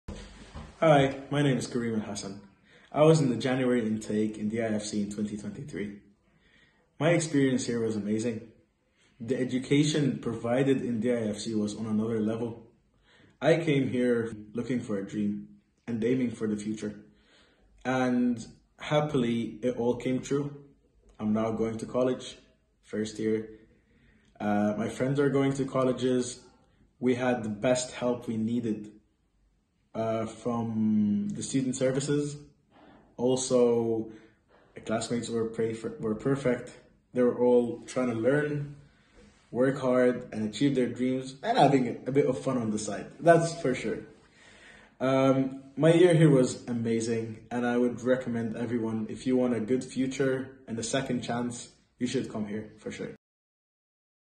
Karim Alhassan, a recent graduate from our institution shares his experience at DIFC
Join us in an enlightening conversation with Karim Alhassan, a recent graduate from our institution. In this video, he shares his valuable insights and experiences from his time at DIFC. 📚

Karim, we extend our heartfelt wishes to you for a future filled with continued success and boundless opportunities! 🌟

Discover the DIFCExperience through the eyes of one of our esteemed alumni. Don't miss out on his inspiring journey and the wisdom he imparts.